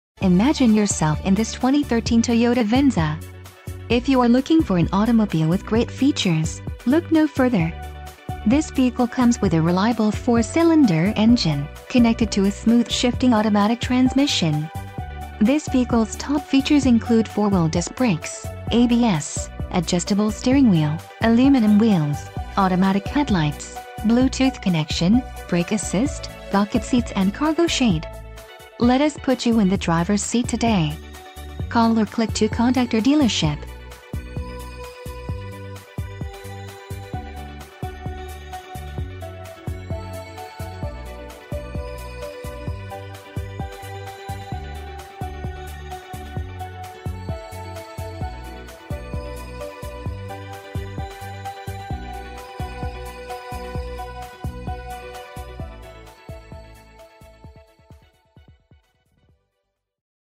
2013 Toyota Venza 4-cylinder LE - Toyota and Scion of Esc...
2013 Toyota Venza 4-cylinder LE Condition: Used Mileage: 36465 Exterior Color: Red Doors: 4 Stock: #51292 Cylinders: 4